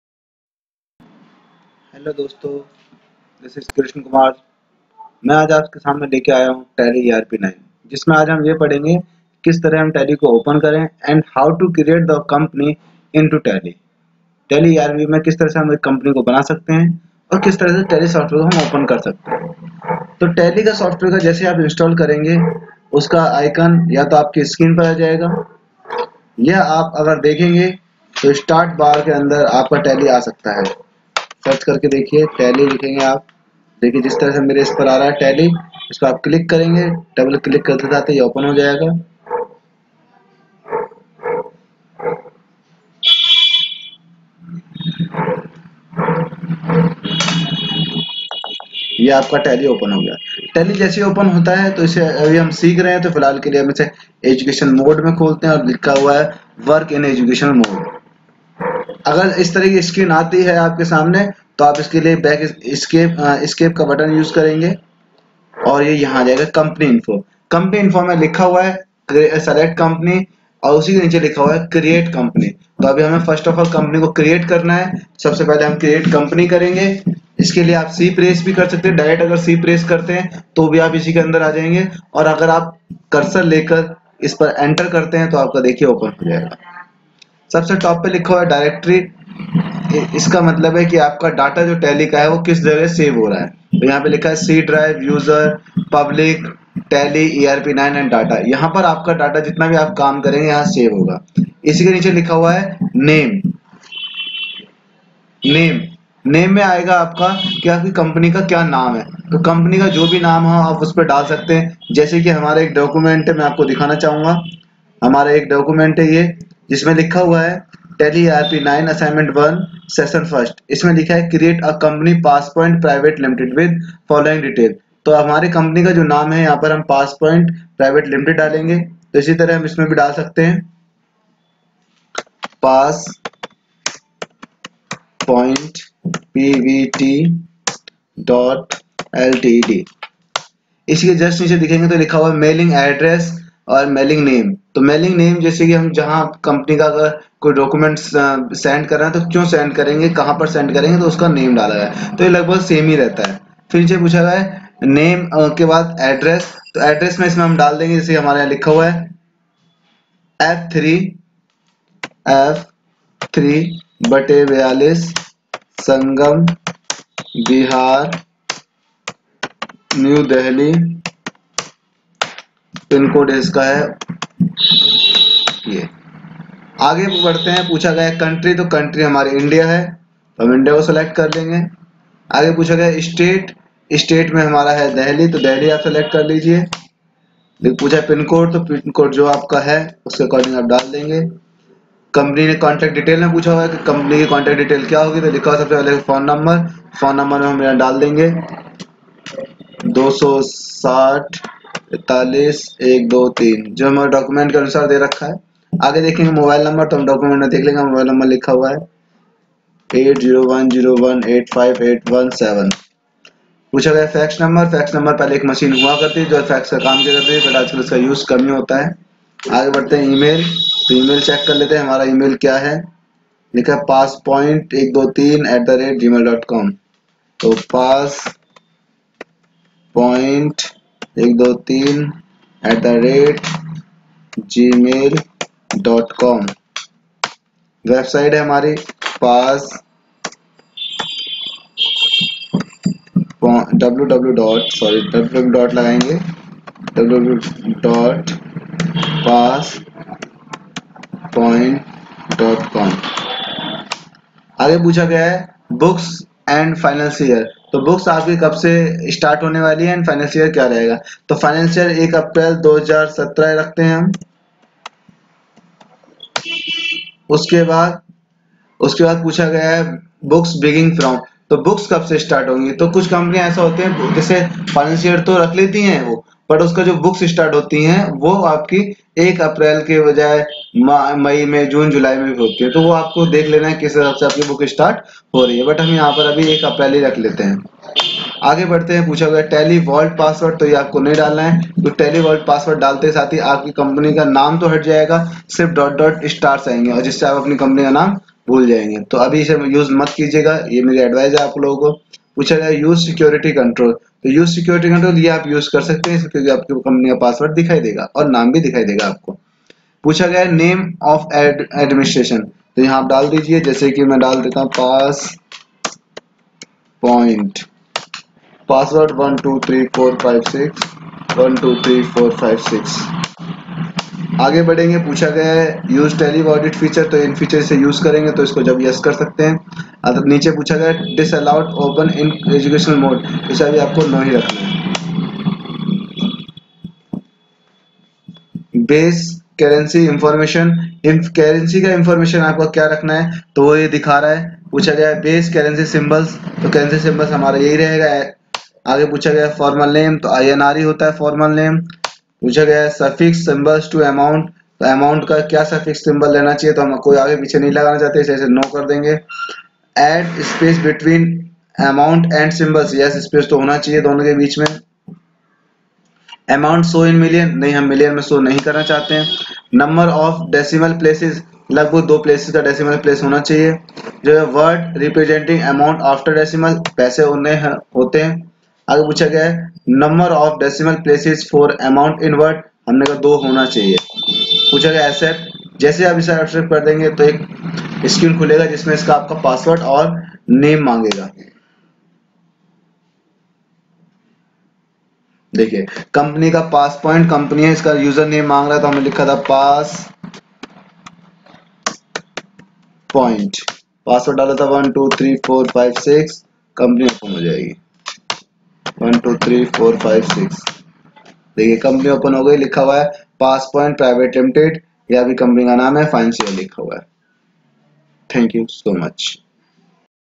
how to create a company in Tally (session 1)
this a very important video regarding creation of TALLY